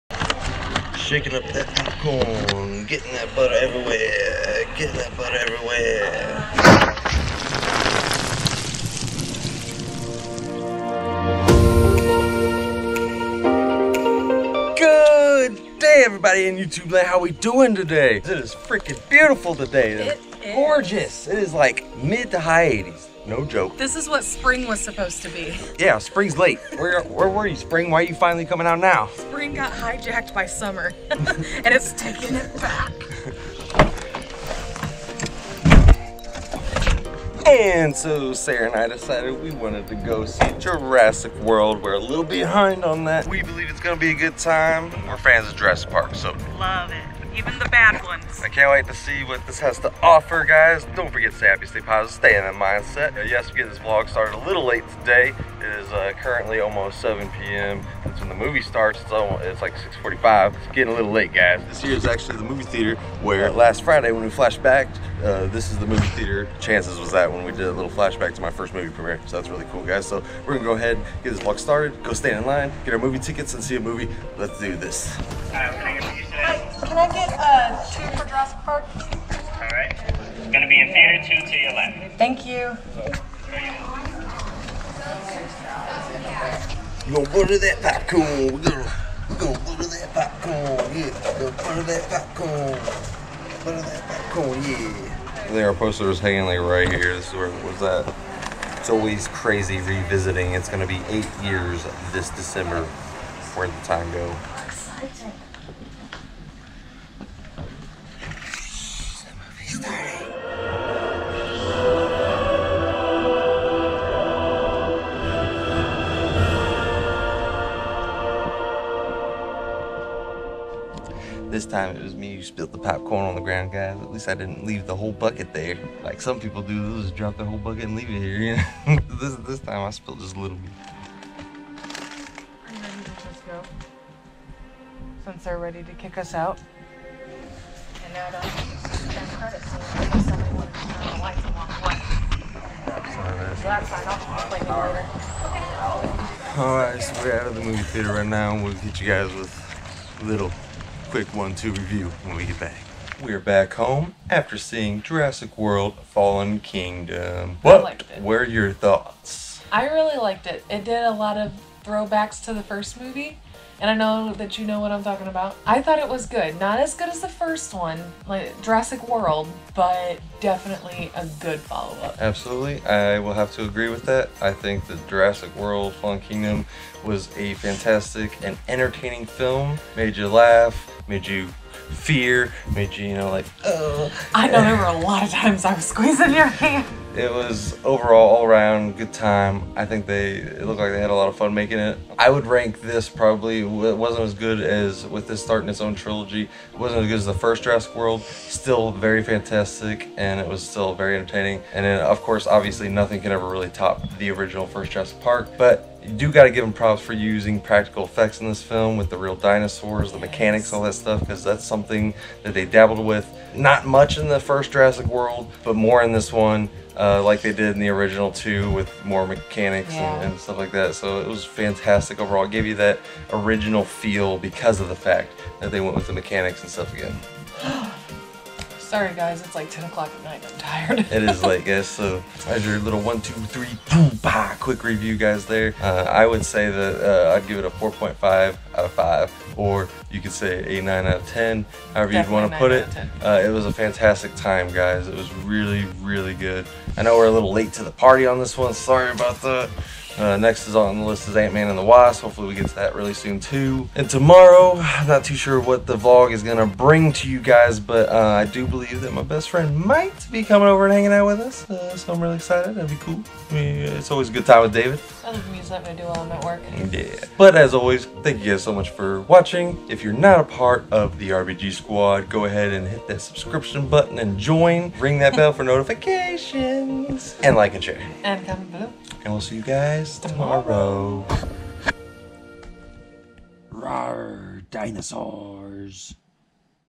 JURASSIC WORLD : FALLEN KINGDOM!
Vlog #37 - In today's vlog, Sara & I get a late start in the Vlog, have a movie/date night out, go to the movies, we see Jurassic World, review it & discuss,  all in one and more! Join us for laughs.

Become apart of the family and join the RBG squad!

Stay Happy. Stay Positive.
Stay in that Mindset!

Your Daily Dose of Entertainment!